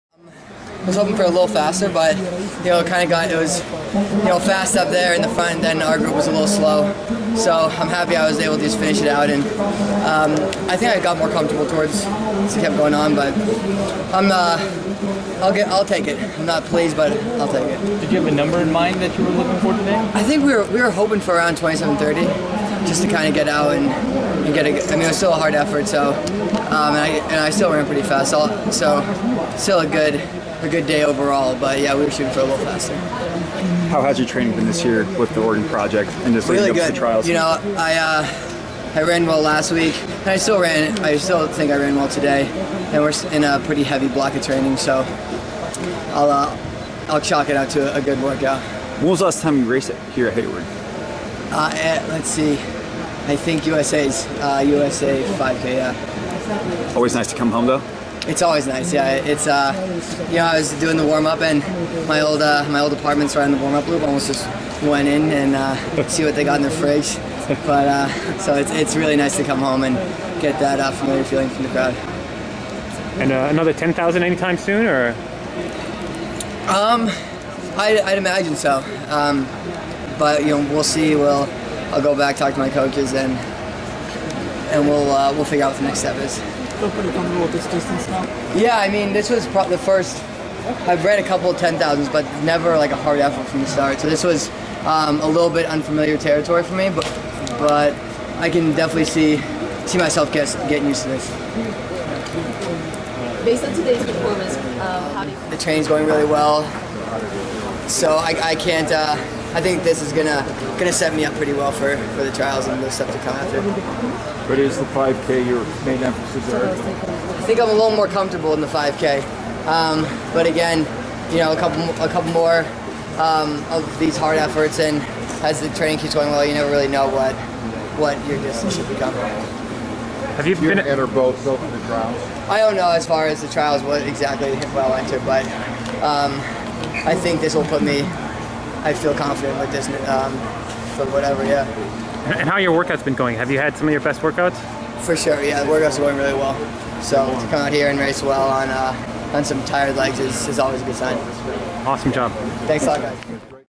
Erick Jenkins 2016 Pre Classic 10,000 m Interview
Erick a former University of Oregon graduate making huge steps in the 10,000m running 27:48 for a new PR over 1 minute.

Eric Jenkins (born November 24, 1991) is a professional distance runner. He first began running at Portsmouth High School in his hometown, after which he was recruited by Northeastern University. After his junior year Jenkins transferred to University of Oregon, where he further established his reputation in collegiate distance running. After his collegiate career he signed a contract with Nike. He currently runs with the Nike Oregon Project.

Personal best(s)
1500 meters: 3:38.98
Mile: 3:57.09
3000 meters: 7:41.79
5000 meters: 13:07.33
10,000 meters: 27:48.02